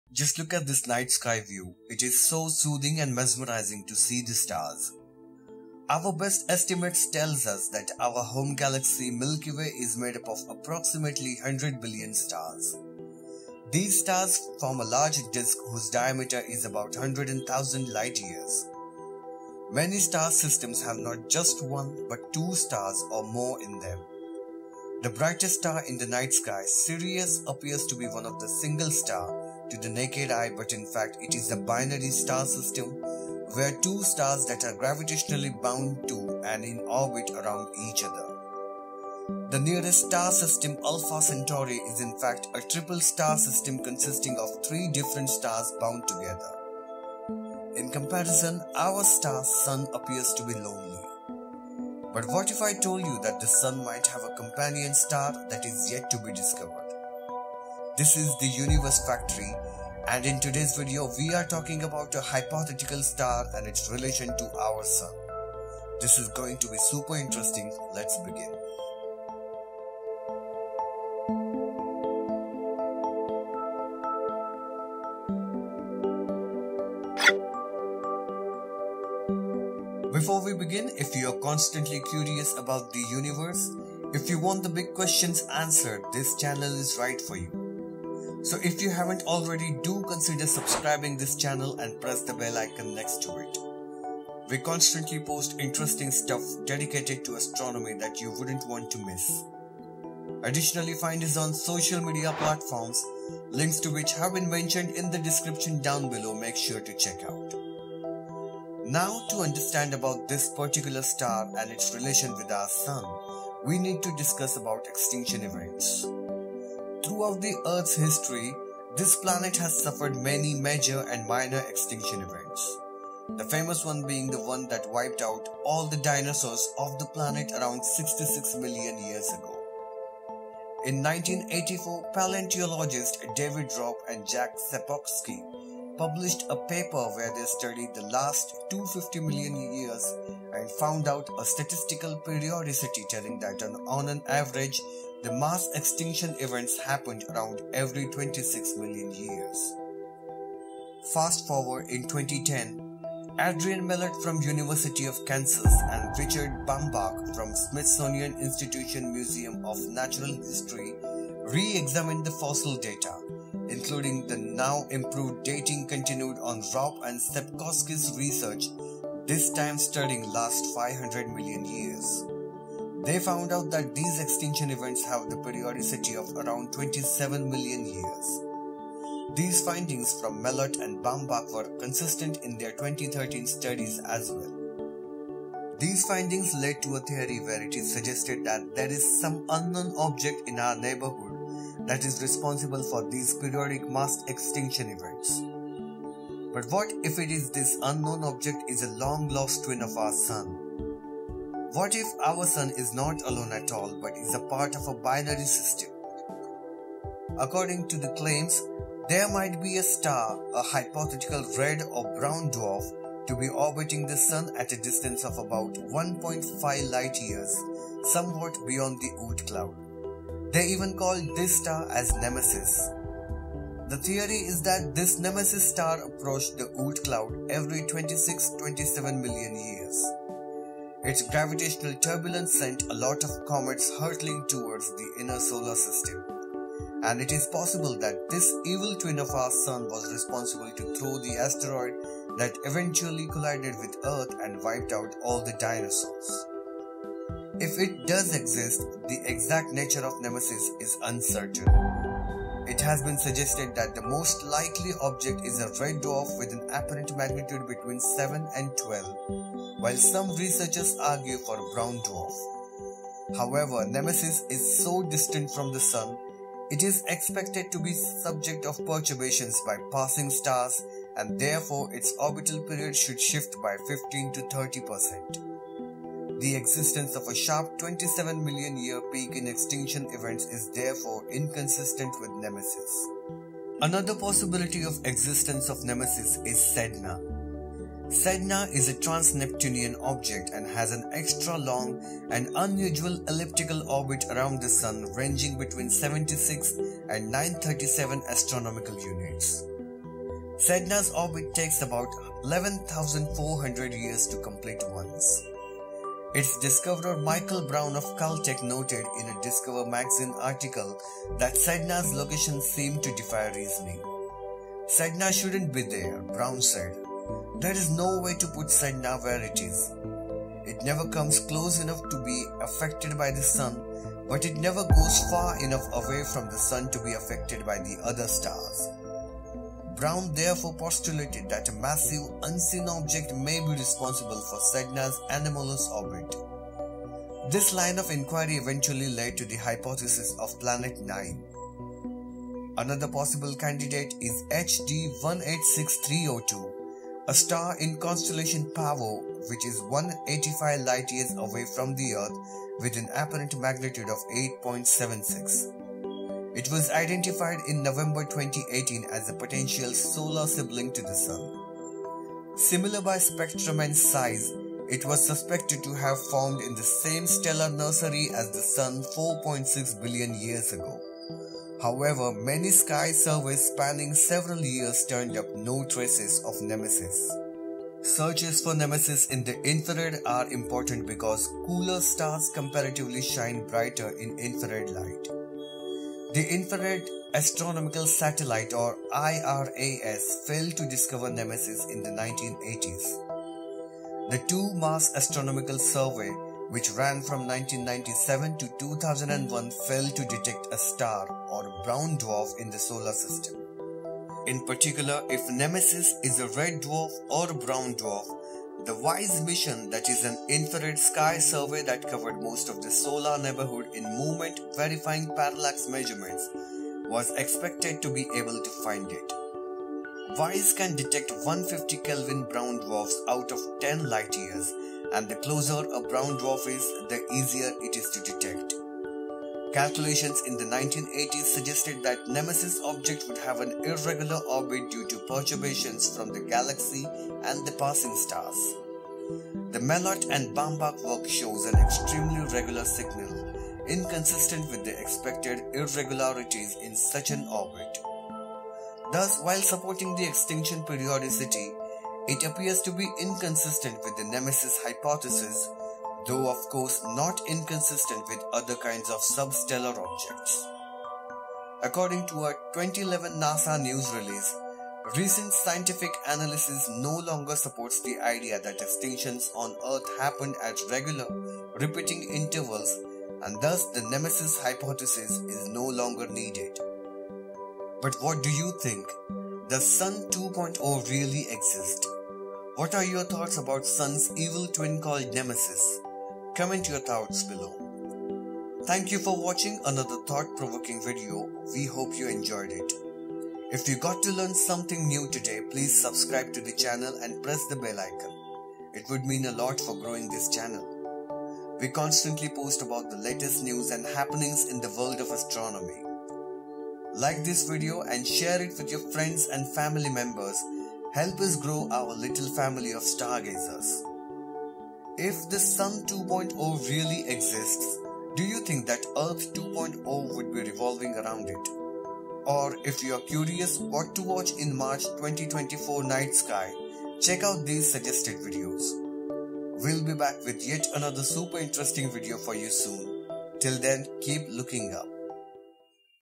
NEMESIS - The Evil Twin of our Sun ?
Have you ever wondered if our sun has a sibling? According to a recent theory, most stars are born in pairs, and our sun might have had a companion that could be responsible for some of the mass extinctions on Earth. This hypothetical star is called NEMESIS, and it is said to orbit far away from the sun, in the outer reaches of the solar system.

Every 26 to 27 million years, Nemesis might disturb the comets and asteroids in the Oort cloud, hurtling them into inner solar system, even sending some of them crashing into our planet.

Could Nemesis be the real culprit behind the demise of the dinosaurs and other life forms? Is Nemesis still out there, waiting for its next encounter with Earth? In this video, we will explore the evidence and the challenges of the Nemesis theory, and what it means for our understanding of the sun and the solar system.